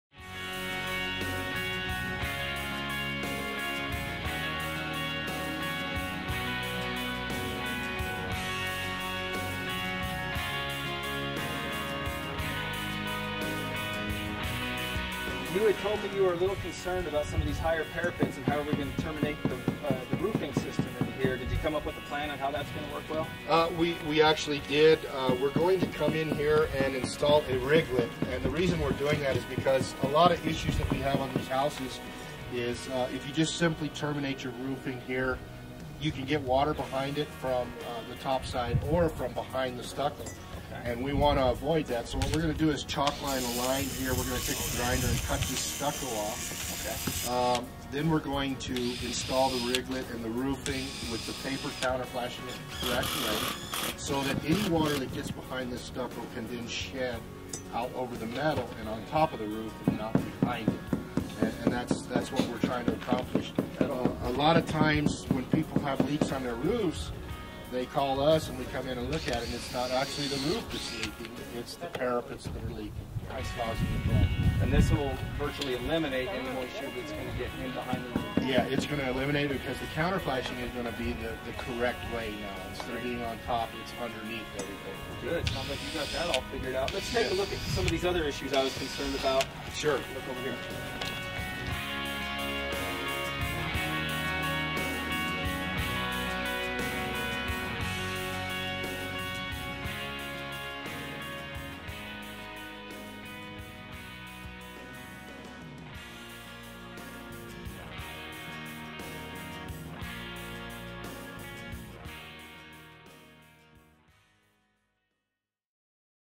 Parapets - Southwest Spray Foam
In this segment, owners Matt Segura and Aaron Lewis of Southwest Spray Foam in Santa Fe, New Mexico discuss the options used to properly seal the PVC roofing  to the internal parapets in this Duro-Last PVC Prefabricated Roofing System installation.